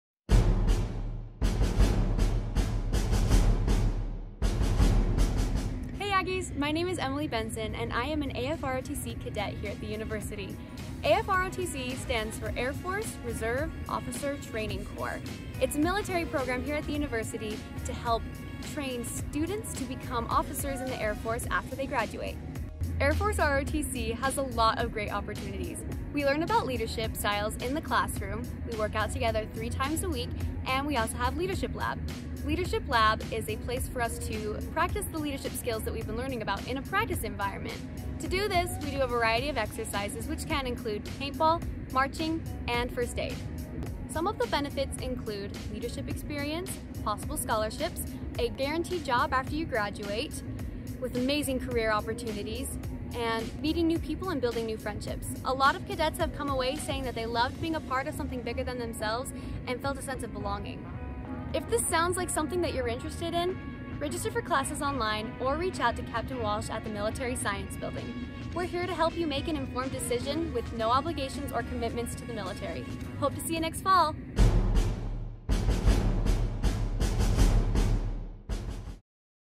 Air Force ROTC at USU
The Air Force Reserve Officer Training Corp (AFROTC) program at USU is a training program that prepares college students to become officers in the United States Air Force. Students work with Detachment 860 to qualify for a commission as an officer in the Air Force, while simultaneously completing their university undergraduate degree requirements.